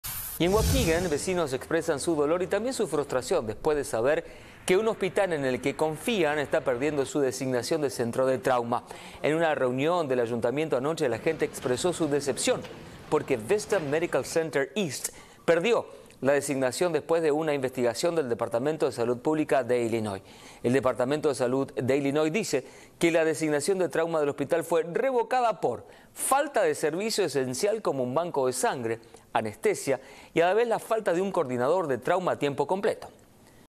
Residentes de Waukegan expresan frustración por cambios en hospital
Residentes de Waukegan expresan dolor y frustración al saber que su comunidad ya no cuenta con un centro de trauma luego de que Vista Medical Center perdiera esa designación por los hallazgos del Departamento de Salud Pública de Illinois en una investigación.